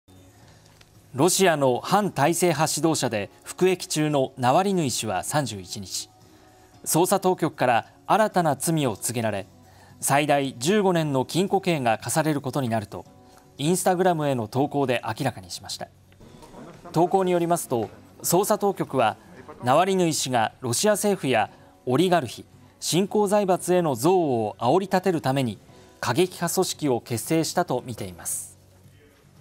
新たな罪で禁錮最大15年か 露反体制指導者【モ－サテ】（2022年6月1日）
ロシアの反体制派指導者で服役中のナワリヌイ氏は31日、捜査当局から、新たな罪を告げられ、最大15年の禁錮刑が科されることになると、インスタグラムへの投稿で、明らかにしました。投稿によりますと、捜査当局は、ナワリヌイ氏がロシア政府やオリガルヒ＝新興財閥への憎悪を煽り立てるために過激派組織を結成したとみています。